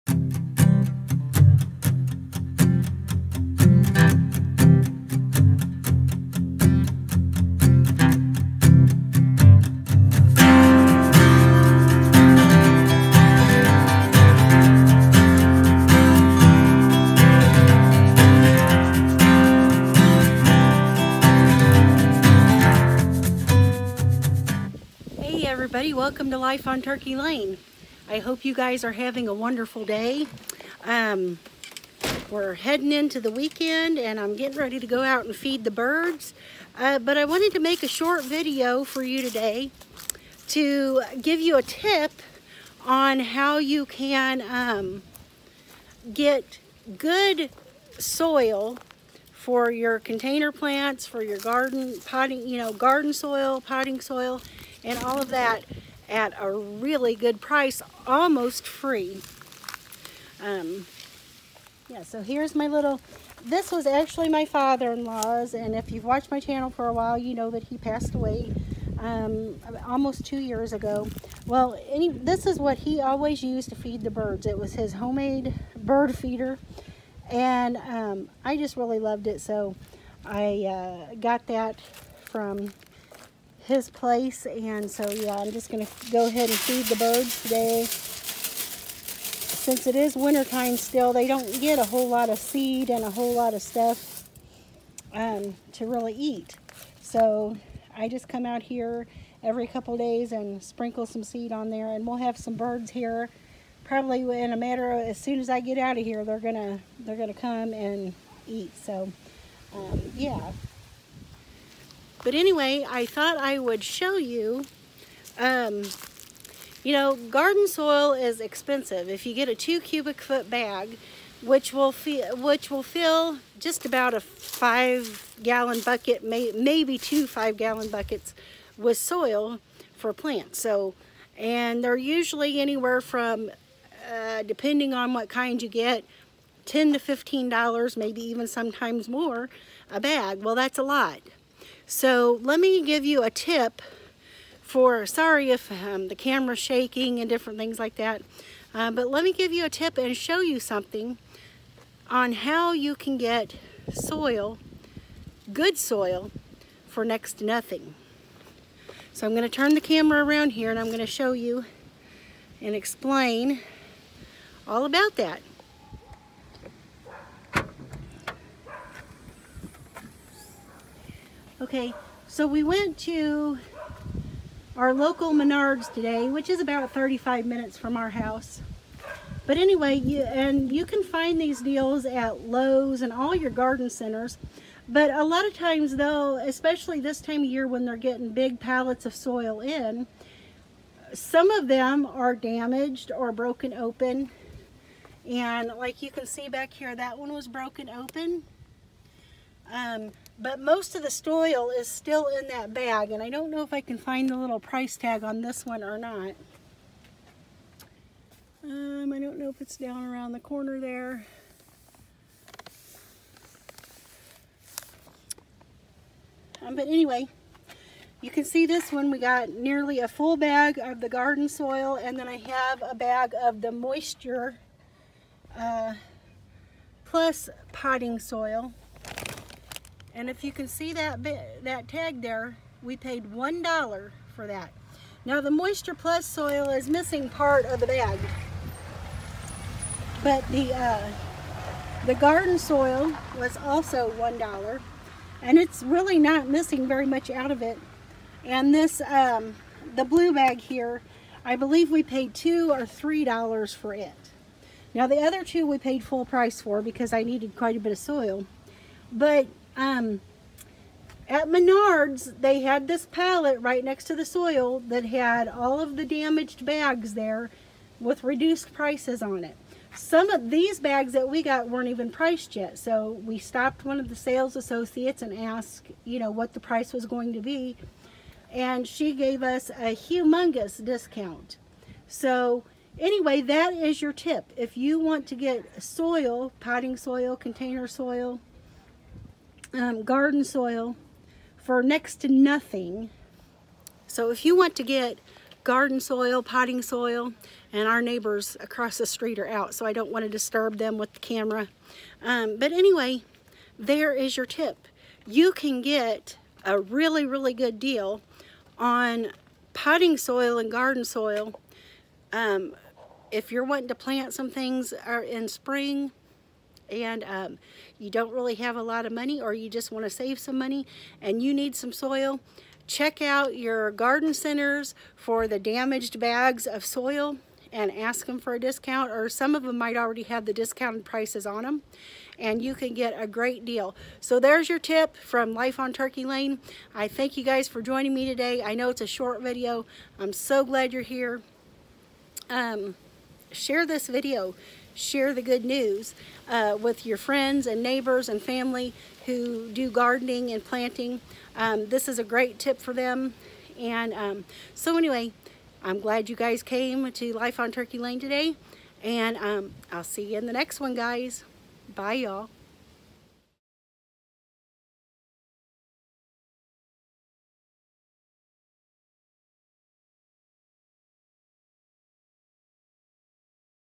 Tip: How to get Soil for Darn Near Free
Right now the garden centers are getting pallets of soil for kick off of the spring planting season. Occasionally a bag or two are damaged or broke open that they can’t sell at full price.  Many times you can buy that soil at a heavily discounted price. You need to RUN to your local garden centers right now to take advantage of this great deal.